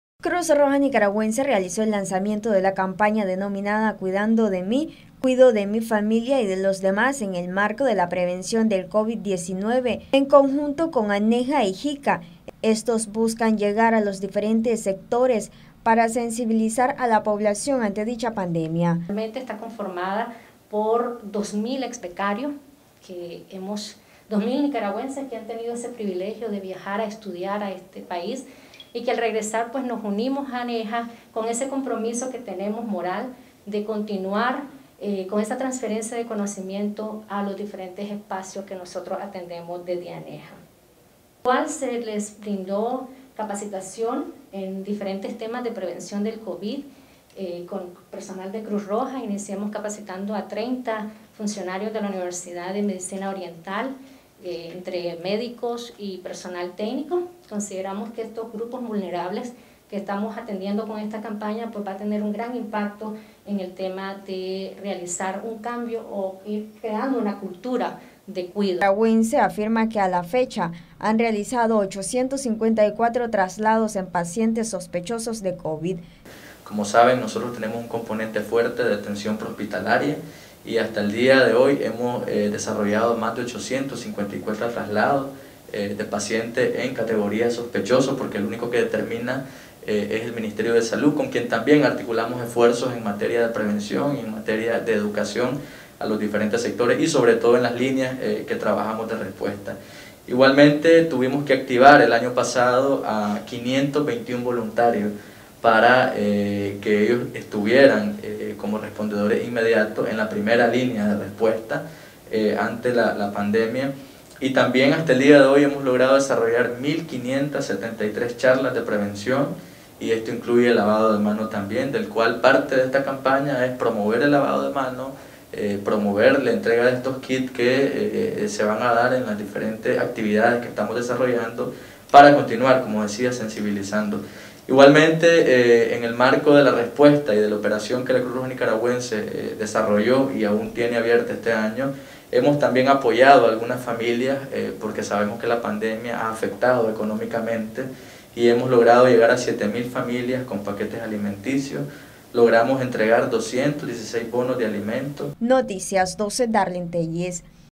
Lanzan campaña de prevención con el fin de sensibilizar a la población ante pandemia
NosResistimosAlEmbargo "Cuidando de mí, cuido de mi familia y de los demás", es la campaña que lanzó Cruz Roja Nicaragüense ante la pandemia del Covid en conjunto con ANEJA y el JICA.